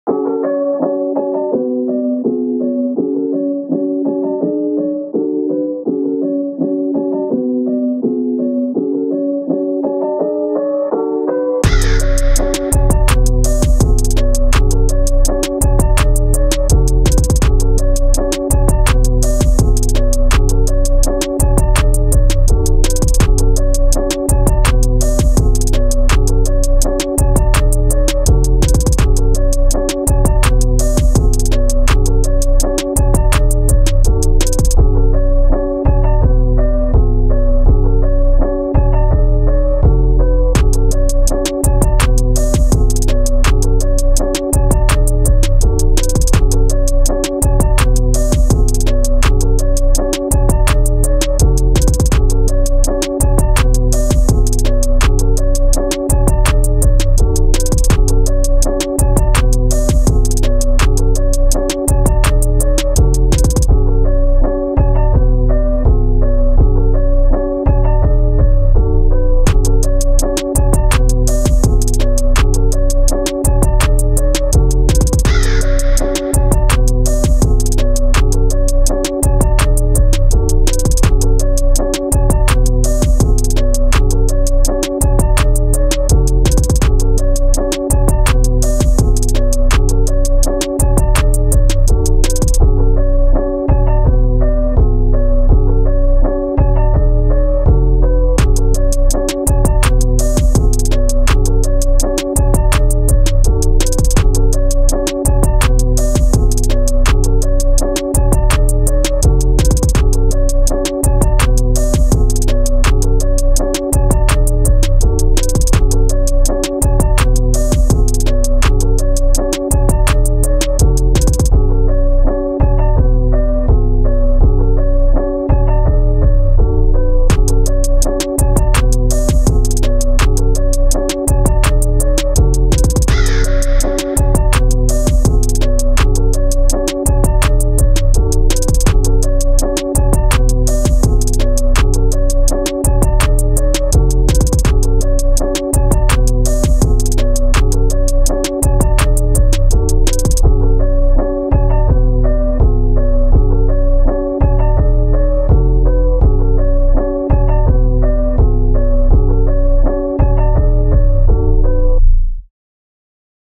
[FREE] Piano Trap Beat | JB | Free Type Beat | Instrumental 2019
Hey Guys

Today I made a paino trap beat
Free 2 use (give credits)

I would Appreciate it if you could leave a like + sub ❤🙏🏼🔥

See ya!